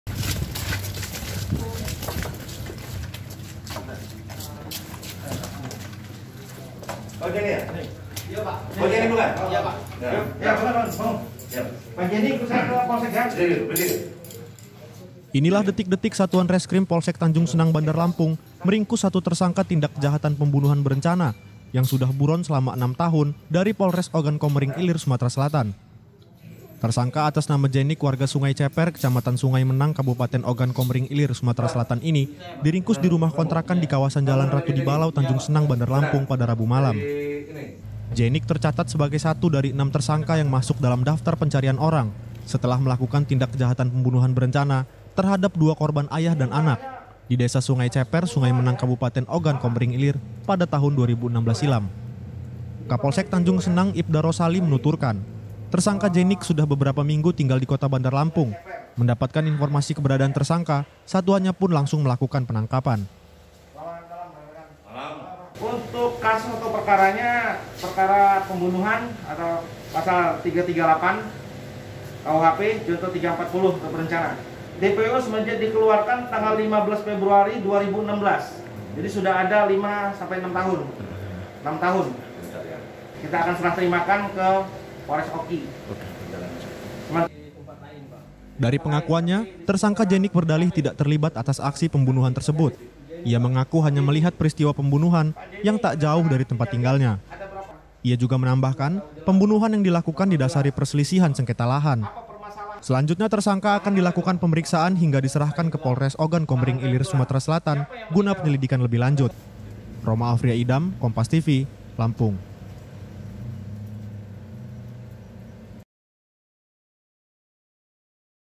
Buron 6 Tahun, Pelaku Pembunuhan Ayah dan Anak ditangkap di Lampung
LAMPUNG, KOMPAS.TV  Jenik (50th) tersangka pembunuhan berencana sudah menjadi buronan Pihak kepolisian dari Polres Ogan Komering Ilir Sumatera Selatan sejak 2016 lalu.

Ia berhasil di ringkus oleh satuan reskrim Polsek Tanjung Senang Bandar Lampung di dalam rumah kontrakan di kawasan jalan Ratu Dibalau,  Tanjung Senang, Bandar Lampung, Rabu (23/6) malam.

Dalam penangkapanya, tersangka yang sedang beristirahat ini tidak melakukan perlawanan kepada pihak kepolisian.

Dalam catatan kepolisian, Jerik merupakan satu dari 7 tersangka yang terlibat dalam kasus pembunuhan berencana yang terjadi di desa Sungai Ceper, Kecamatan Sungai Menang kabupaten Ogan Kemering Ilir, Sumatera Selatan.

Peristiwa pembunuhan terjadi pada tahun 2016 silam, dimana para tersangka telah merencanakan membunuh korban yang merupakan ayah dan anak, lantaran dipicu perselisihan lahan.

Setelah diamankan, jerik yang diketahui sudah beberapa minggu tinggal di Kota Bandar Lampung ini akan diserahkan ke Polres Ogan Komering Ilir, Sumatera Selatan, guna penyilidikan lebih lanjut.

Saat dimintai keterangan oleh petugas, Jerik mengaku tidak terlibat dalam kasus pembunuhan, ia berdalih hanya turut menyaksikan pembunuhan yang dilakukan tak jauh dari kediamanya.

Kepada polisi Jerik tidak merasa kabur, selama ini ia tinggal di kota Bandar Lampung untuk mengurus keperluan sekolah anaknya.